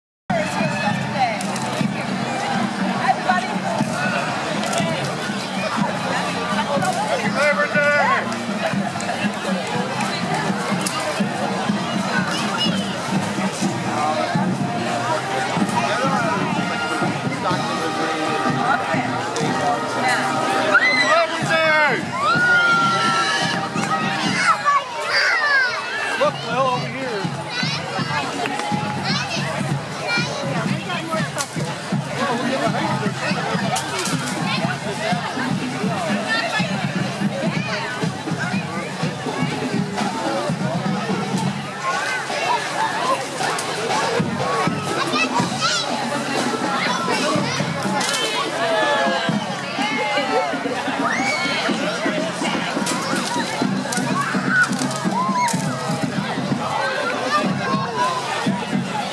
Laborers' Local 288 from Westmont in Labor Day parade
Members of Laborers' Local 288 in Westmont walk along the parade route handing out candy in the Naperville Labor Day parade